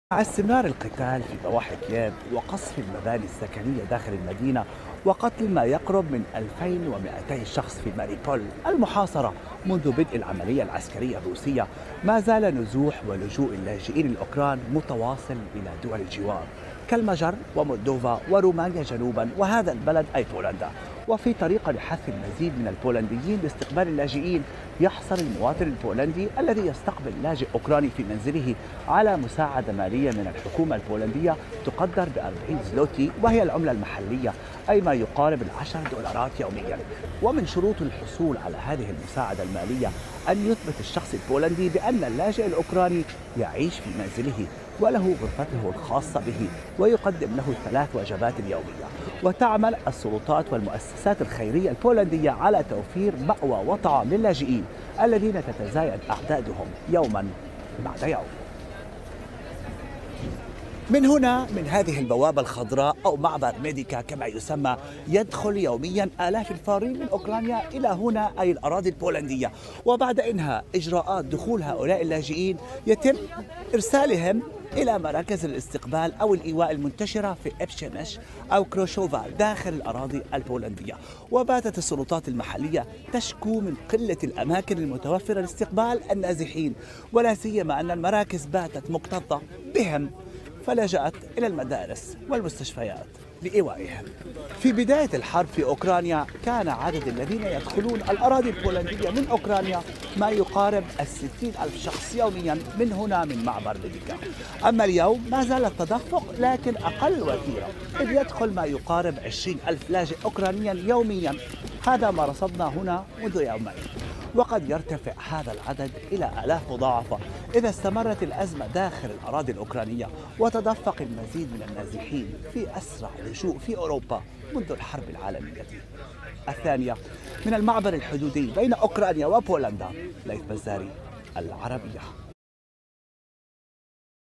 Ukraine Poland Border War in Ukraine Refugees
Ukraine Poland Border War in Ukraine Refugees  - Laith Bazari - Al Arabiya TV

 الحرب في اوكرانيا - الحدود البولندية الاوكرانية - لجوء - ليث بزاري - قناة العربية